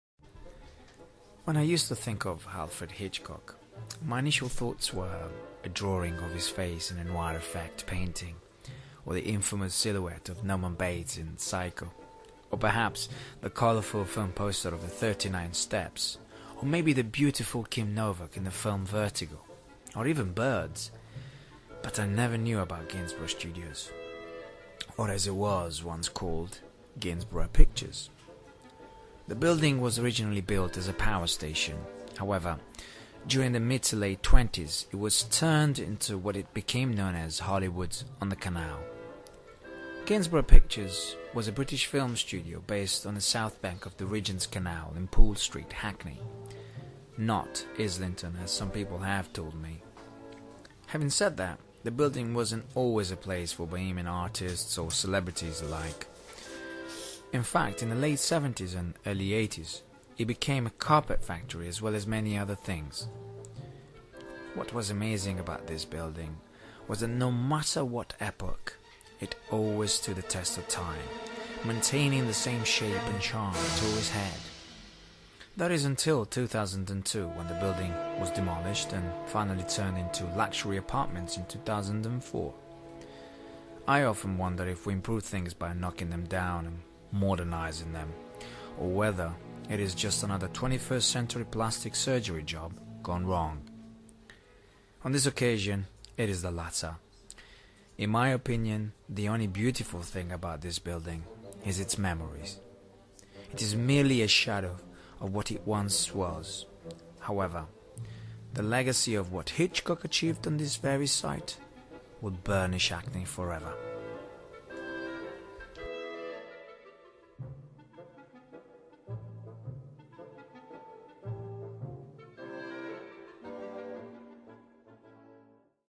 Hollywood on the Canal
A Digital Story by Luca Zizzari about Gainsborough Studios